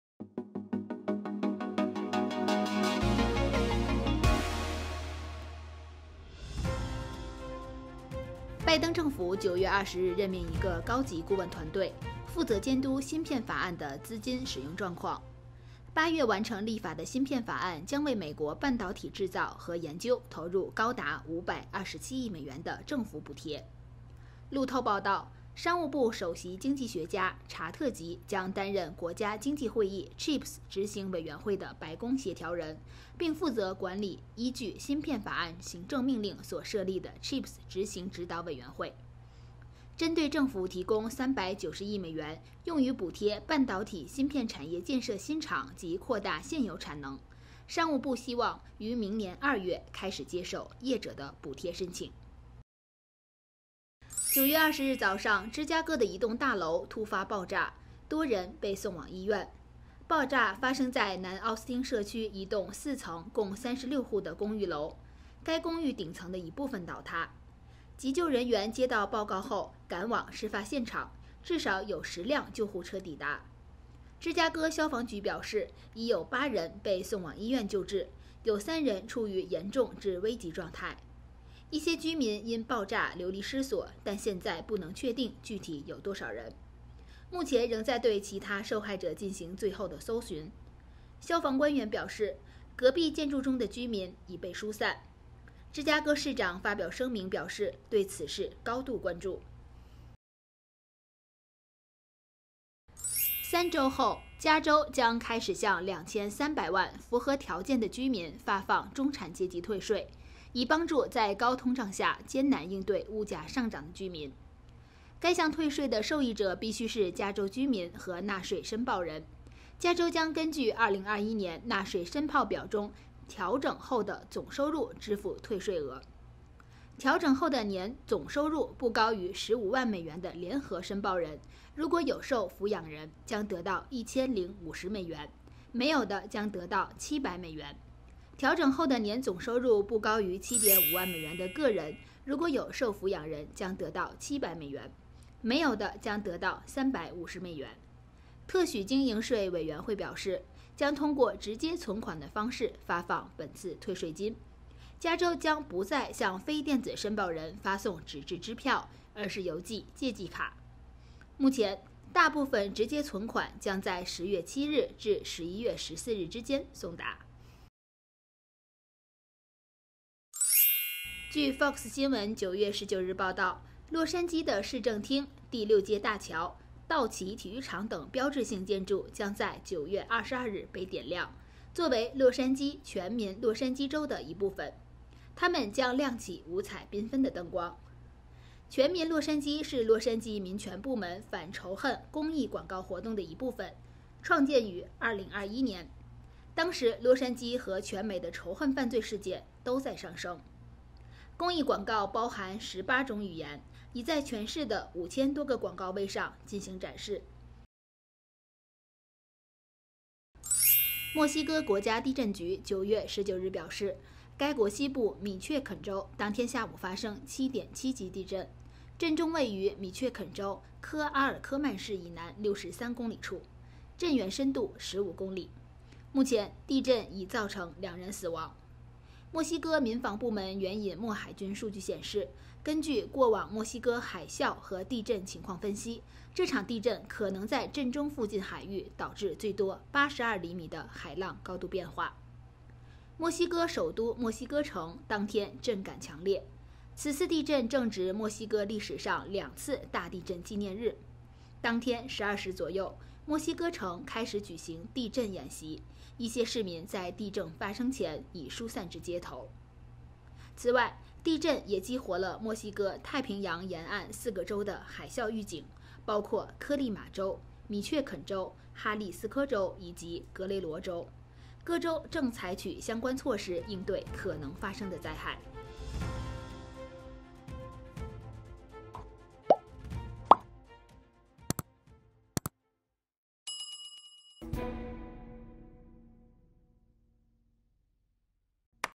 加州即将开始向中产阶级退税; 洛杉矶启动首届“全民洛杉矶”周 以打击仇恨犯罪
00:00  片头

00:06  拜登成立独立团队 监督芯片法案巨额补贴使用

00:53  芝加哥一公寓爆炸 已有8人送医

01:42  加州即将开始向中产阶级退税

02:50  洛杉矶启动首届“全民洛杉矶”周 以打击仇恨犯罪

03:30  墨西哥西部发生7.7级地震